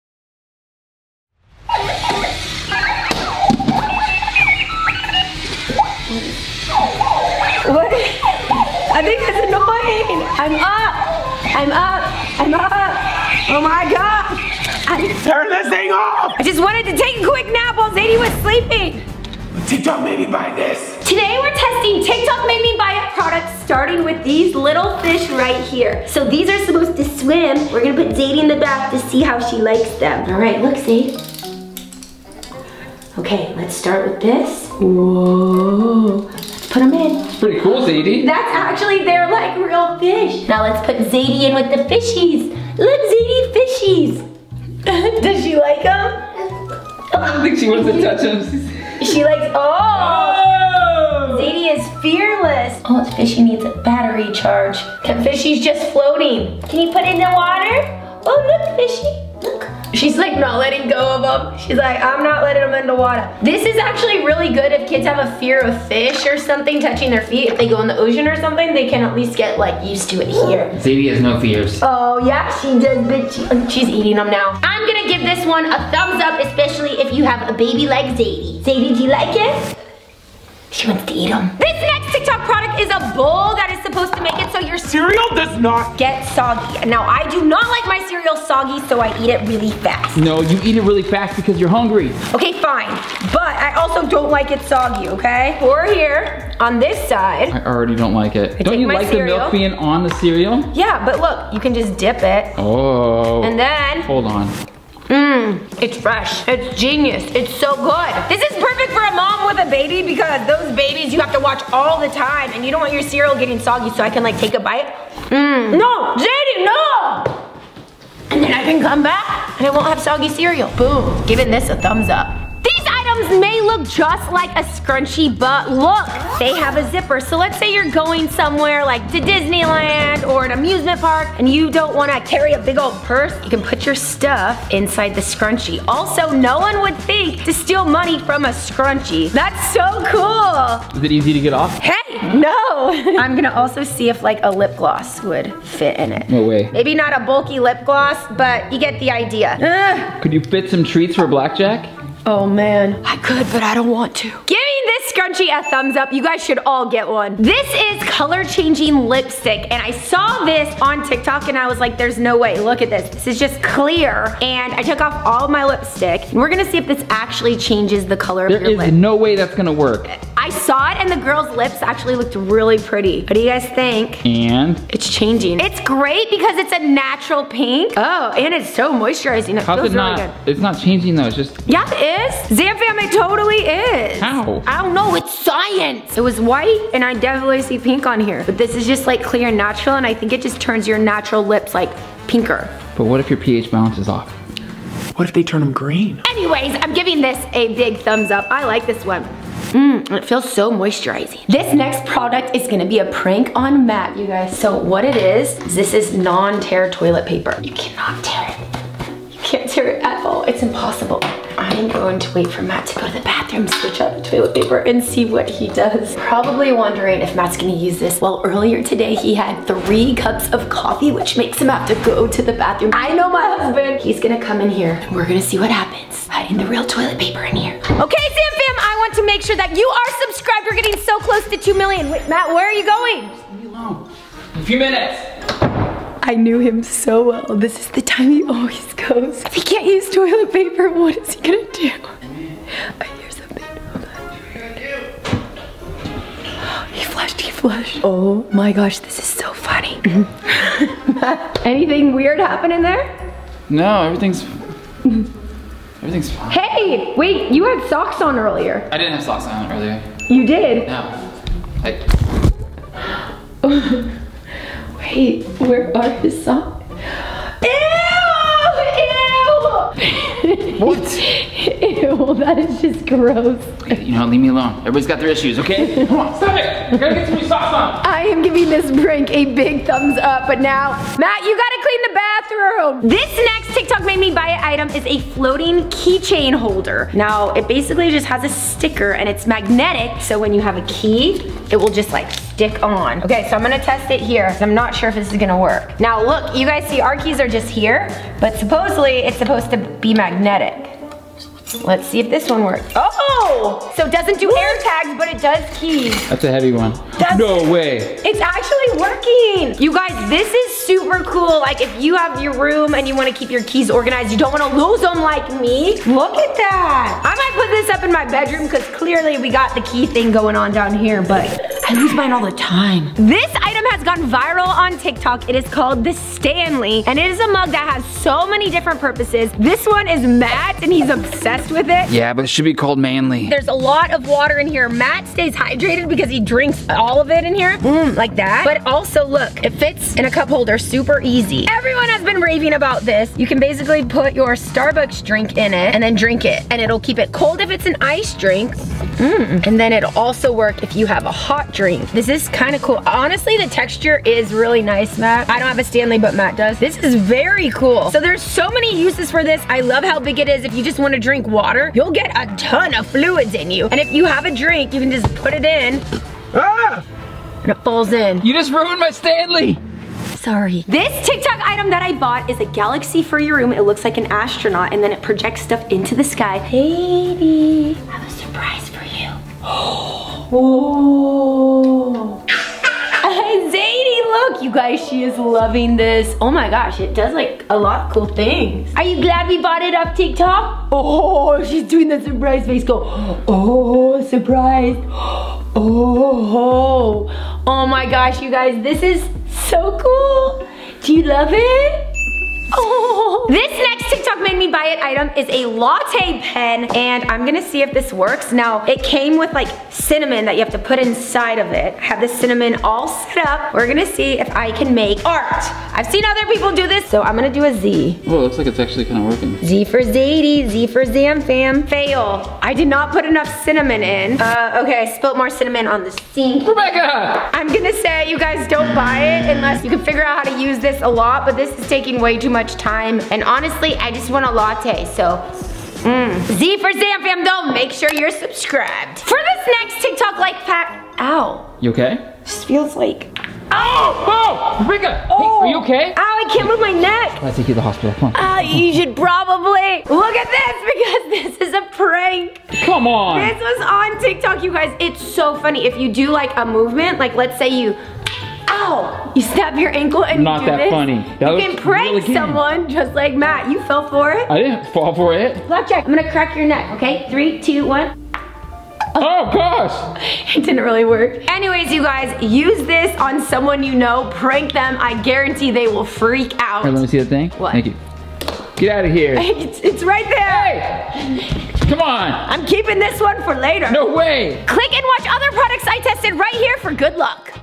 I Tested Tik Tok Made Me Buy It Products
Rebecca Zamolo tests viral Tik Tok made me buy it products to see if they actually work.